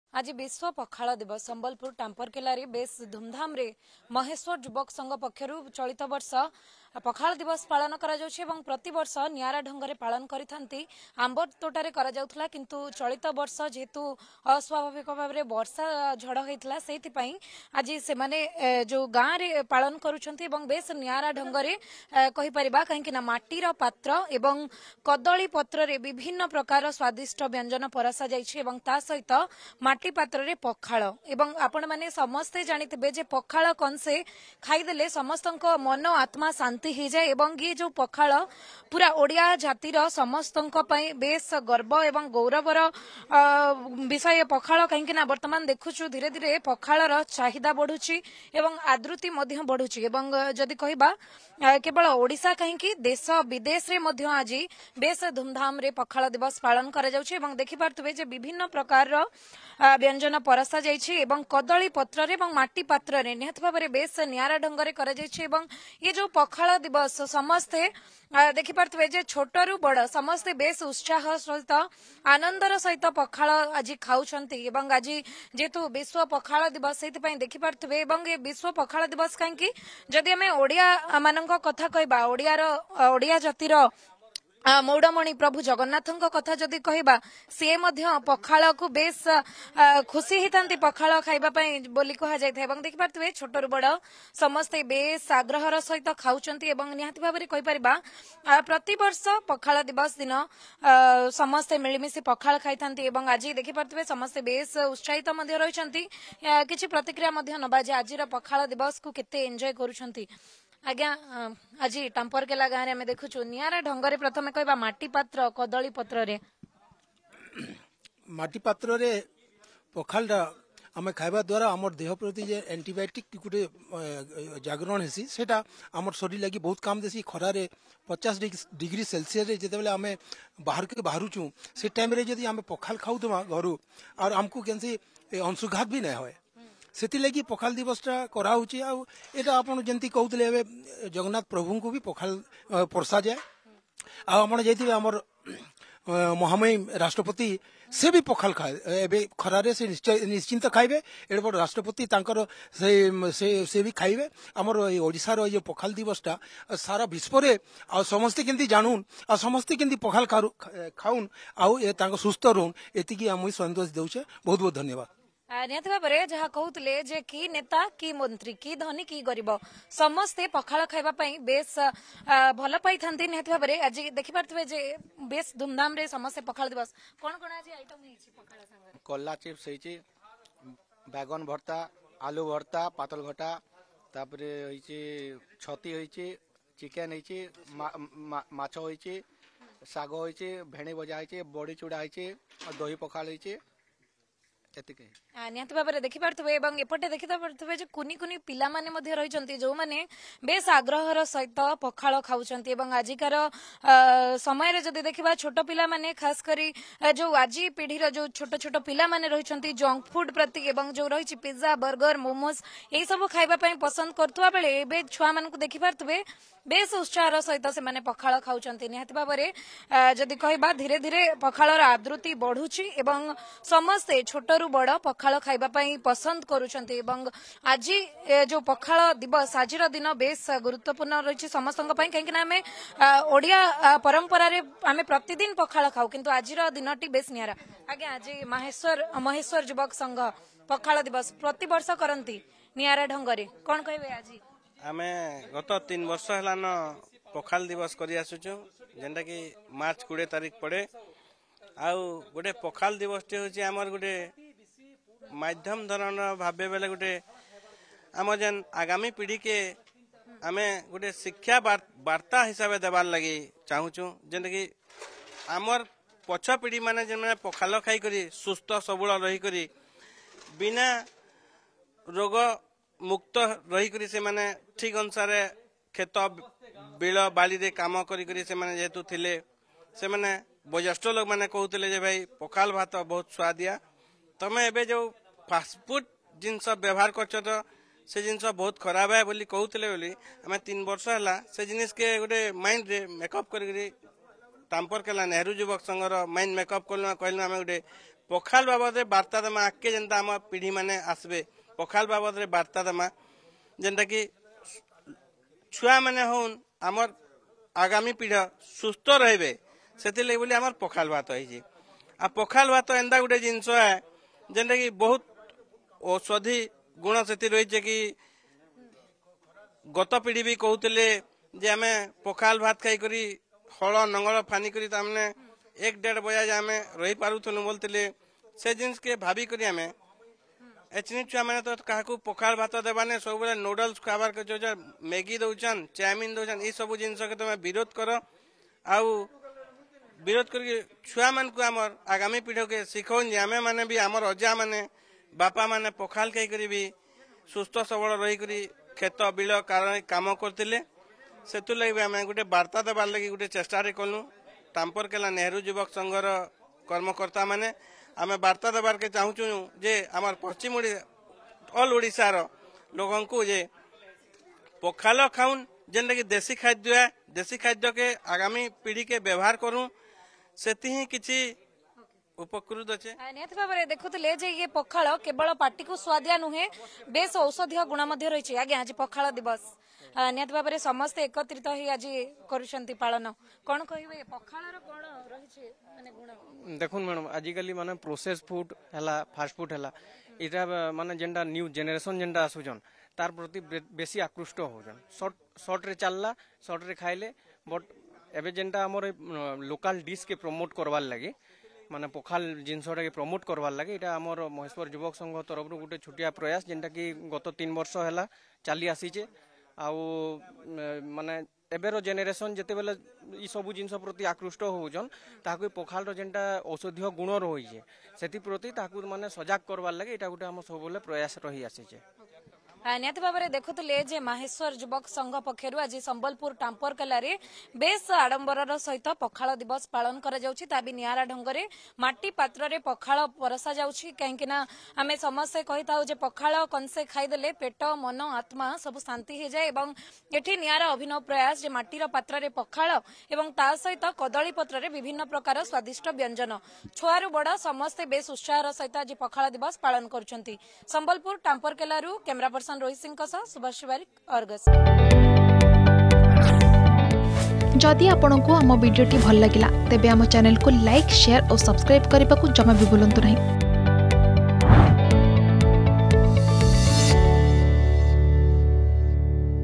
ସମ୍ବଲପୁରରେ ବେଶ୍ ନିଆରା ଢଙ୍ଗରେ ବିଶ୍ୱ ପଖାଳ ଦିବସ ପାଳନ କରାଯାଇଛି || Sambalpur
ଆଜି ପଖାଳ ଦିବସ । ଓଡ଼ିଶାର ପାରମ୍ପରିକ ଖାଦ୍ୟ ହେଉଛି ପଖାଳ । ସମ୍ବଲପୁର ଟାମ୍ପରକେଲାରେ ବେଶ୍ ନିଆରା ଢଙ୍ଗରେ ବିଶ୍ୱ ପଖାଳ ଦିବସ ପାଳନ କରାଯାଉଛି।